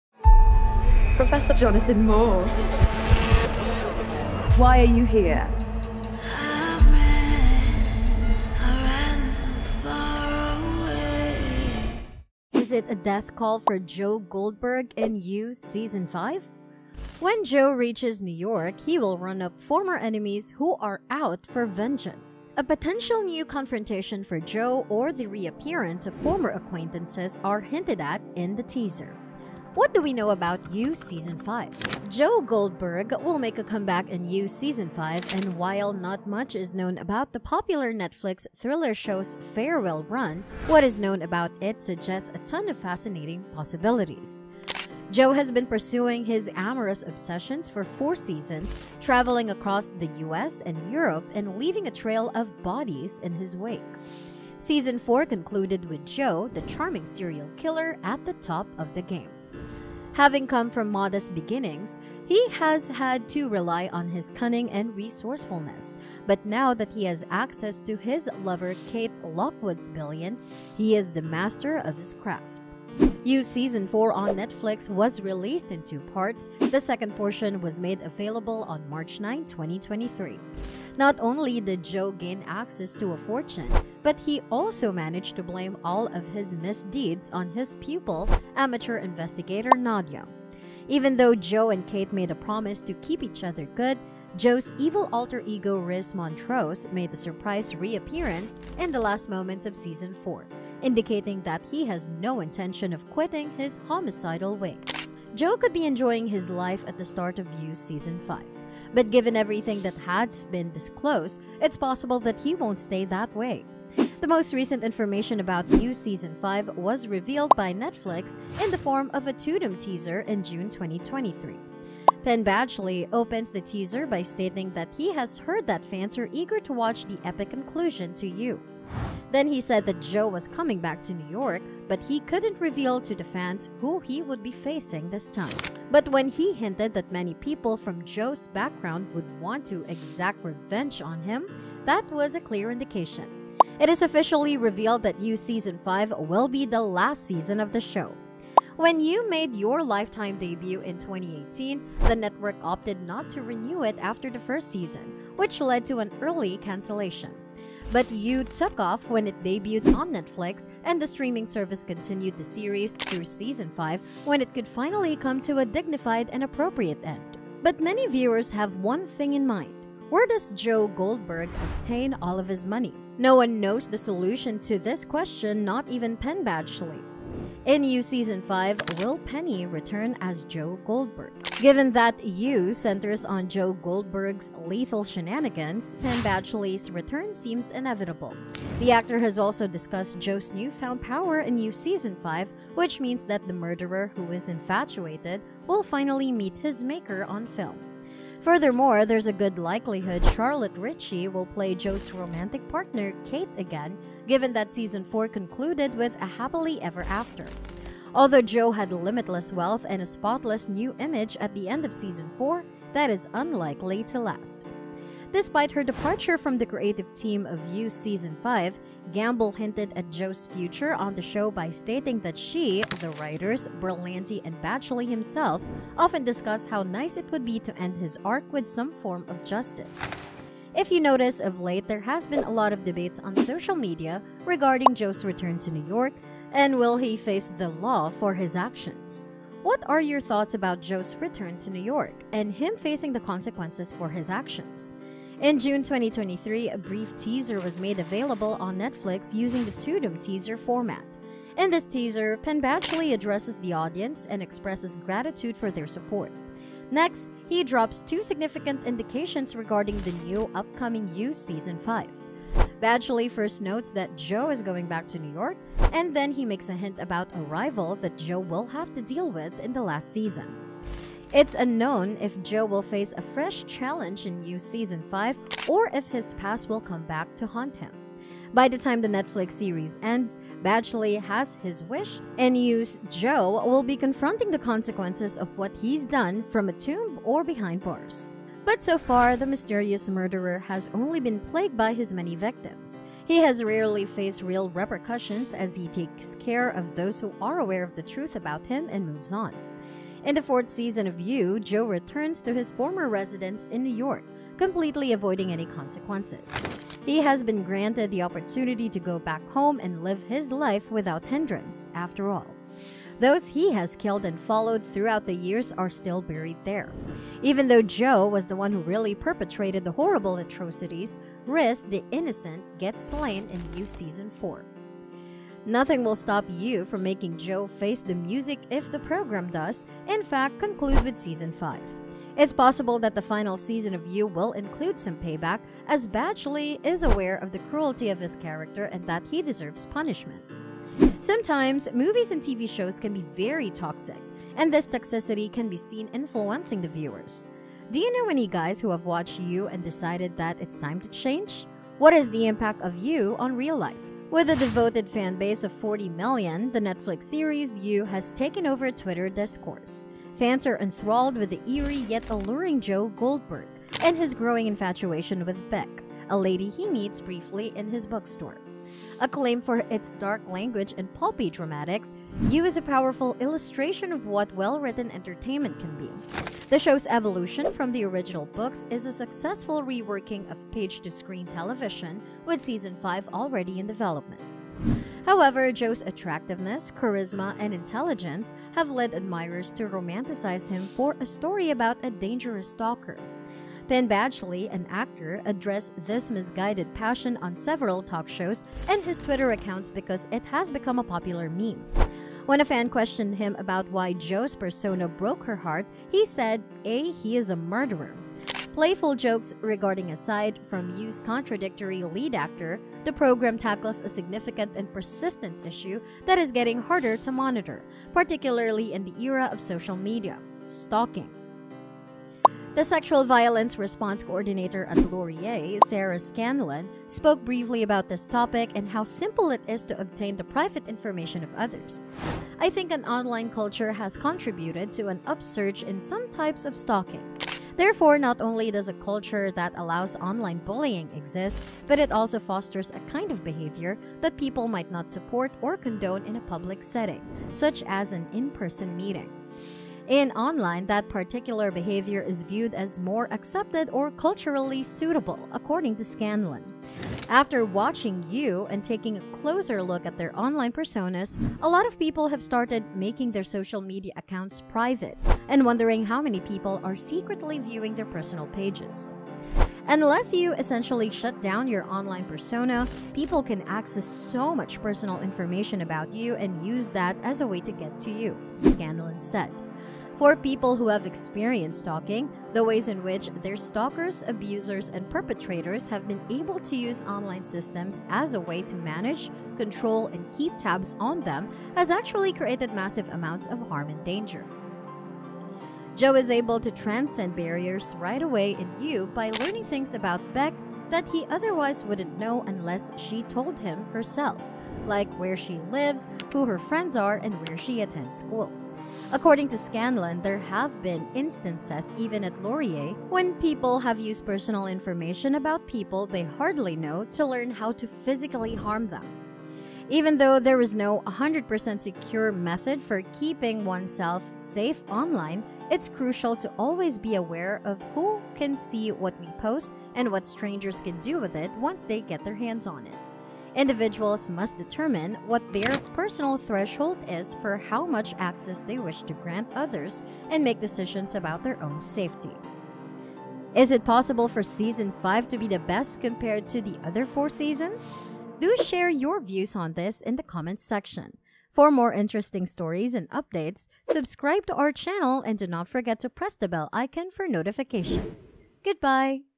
Everything we know about YOU Season 5
Welcome back to EnigmaEntertainTV, today on the channel we're going to be talking about Everything we know about YOU Season 5. If you're excited to learn more about this then make sure you watch the whole way through because you don't want to miss the details we have for you!